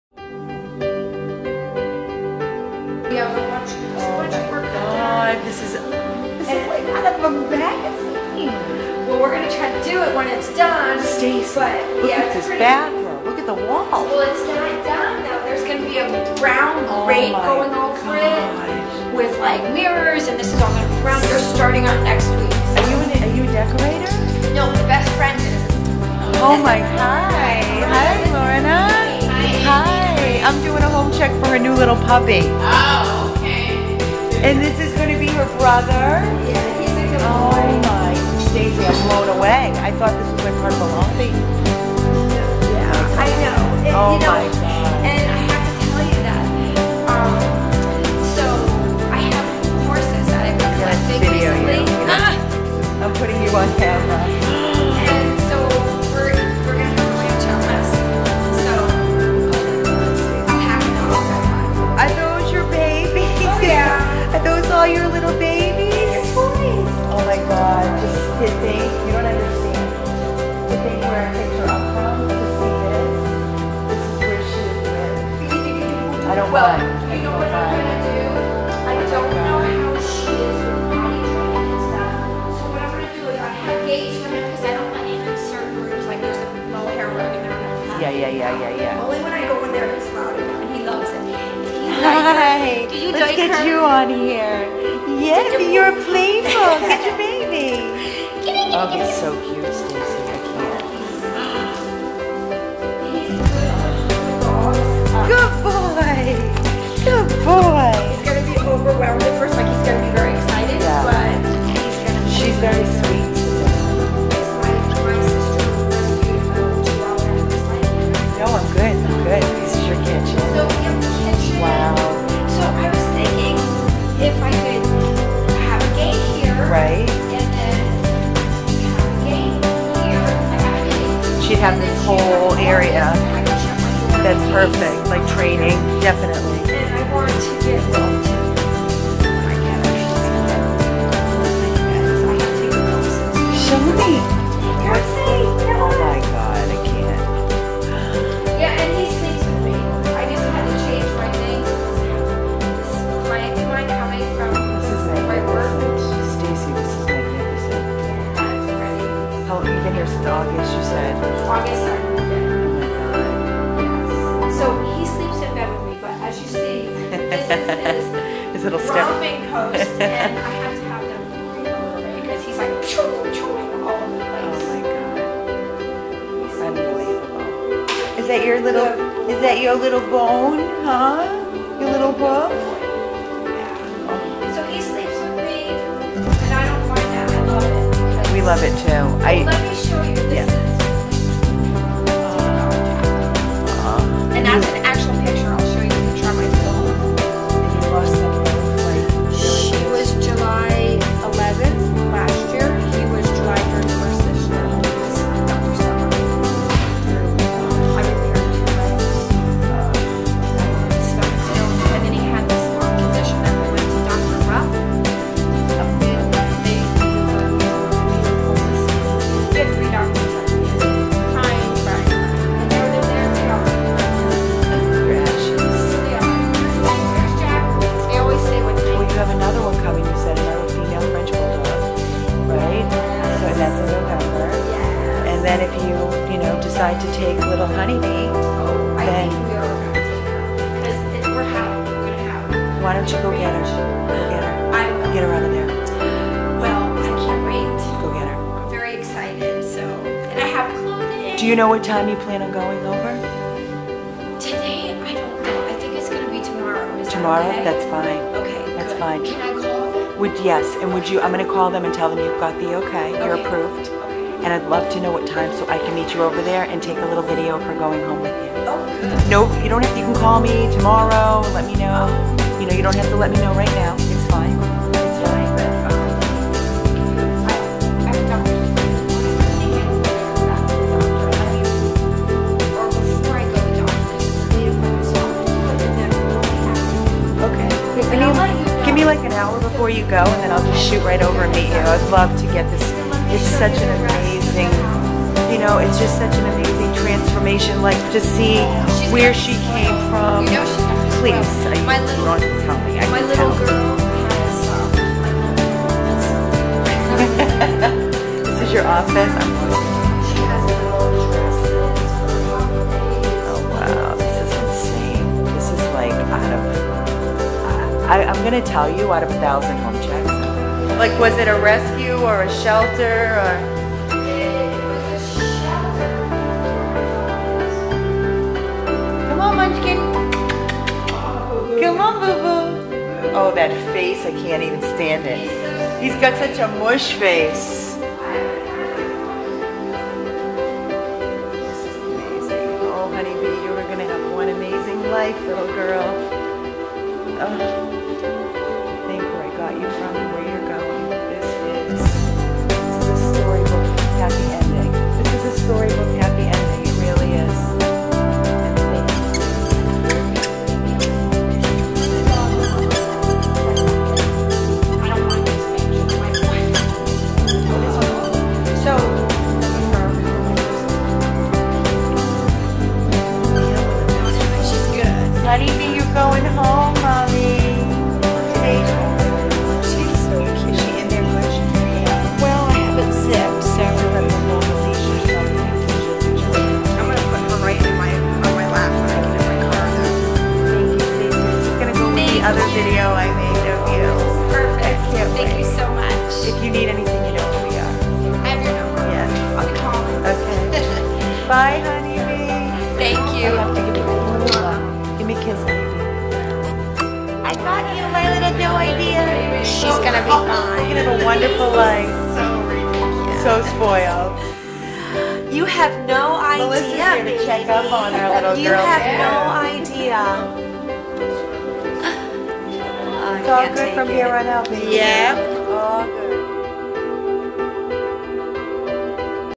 Little Honeybea saved from the unimaginable ADOPTED! Thank you Stacy
Honeybea one of 15 rescued from the most unimaginable living conditions. Spayed, vetted and now in her forever loving home. This little dog will only know love from hear on out.. Thank you for all your love & support..
100 + Abandoned dogs of Everglades Florida.